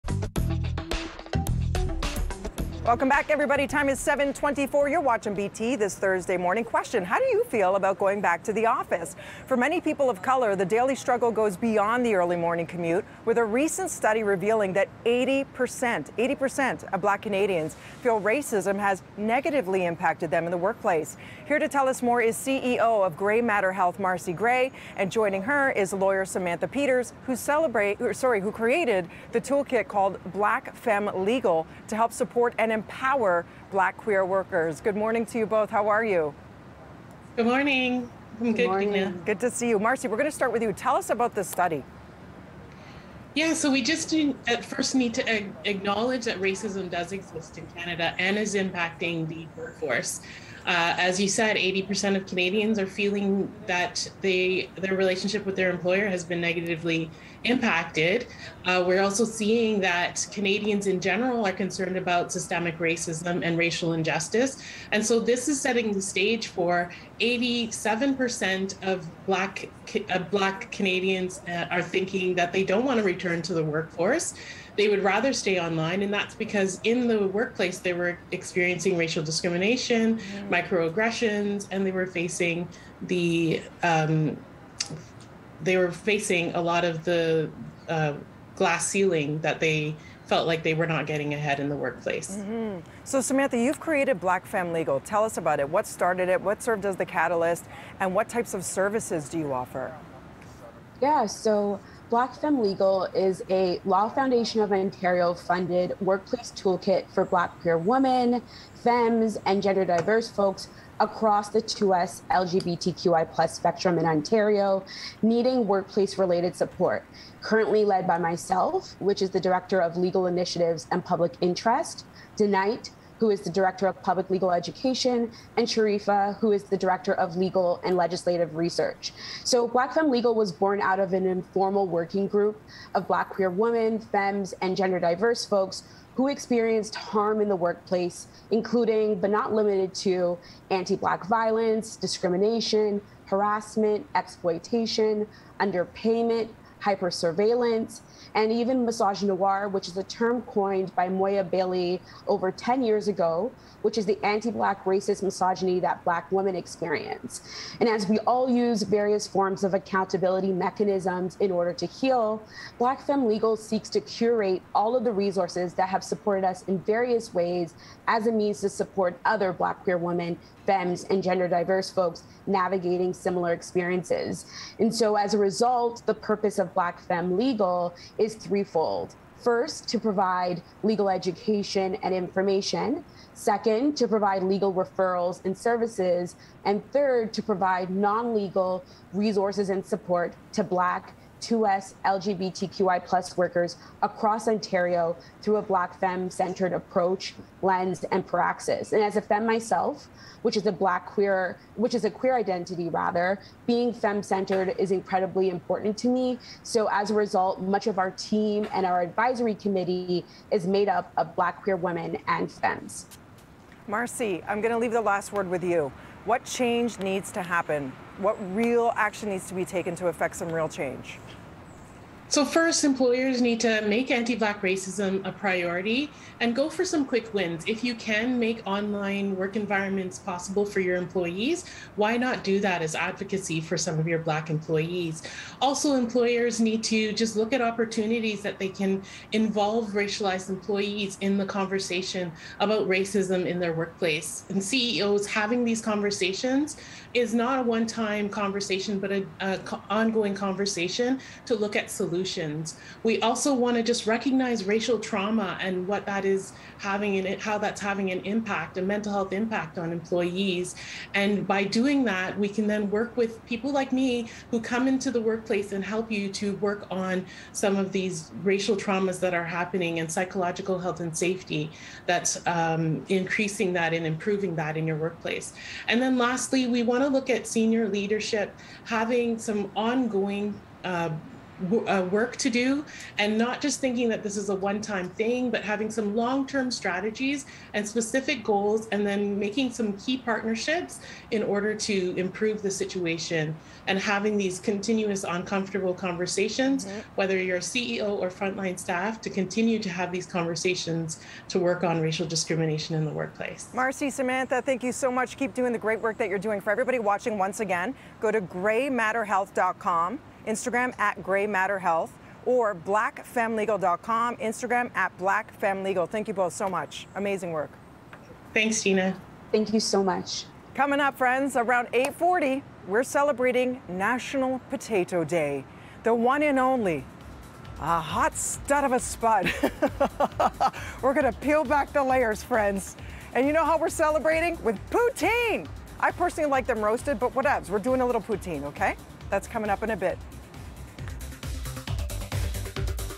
Combating racism in the workplace
A recent study has revealed that 80 per cent of Black Canadians feel racism has negatively impacted them in the workplace. Here to tell us more is CEO of Gray Matter Heath, Marci Gray, and lawyer Samantha Peters.